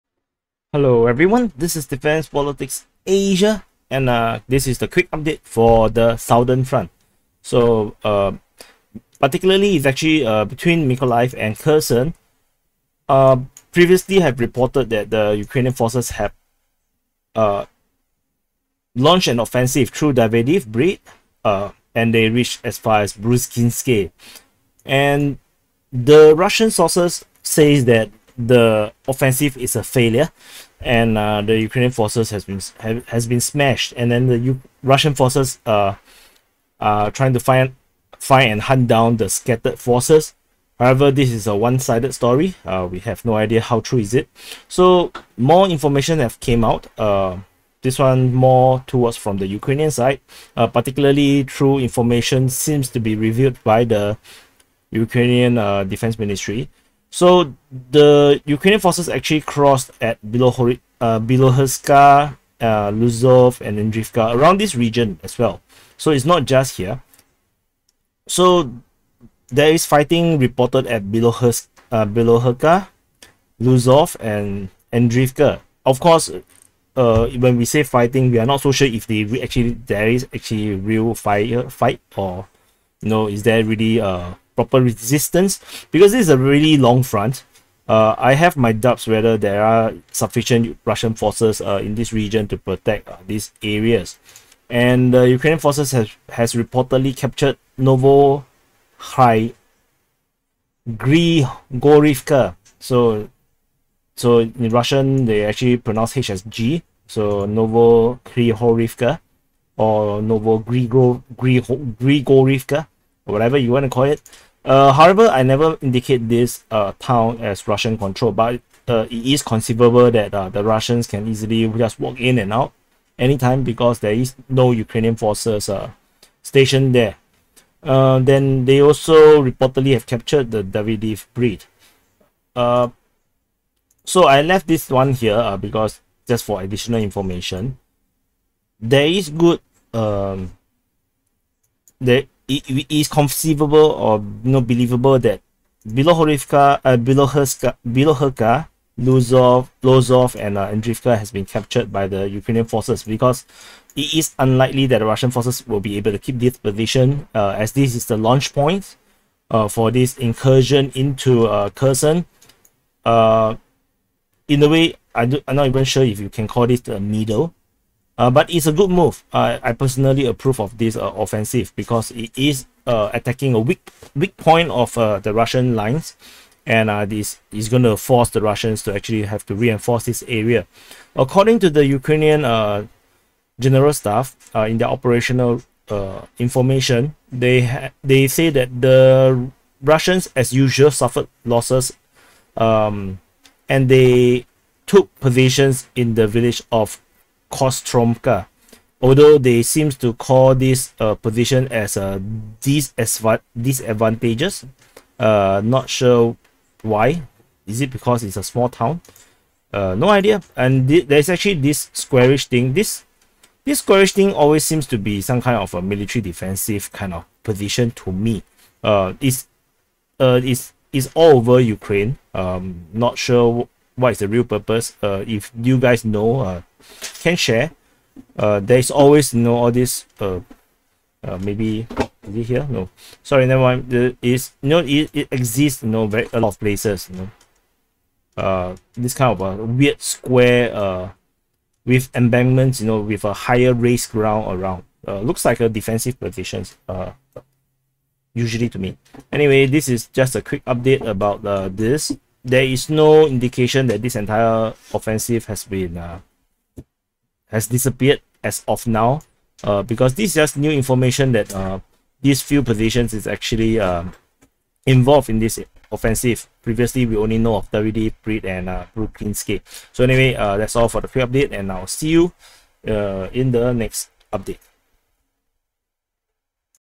[ Southern Front ] Ukrainian forces captured Novohryhorivka & Davydiv Brid - more intel revelations
You can find more information and the interactive map used in the video

Be a DPA Insider!!!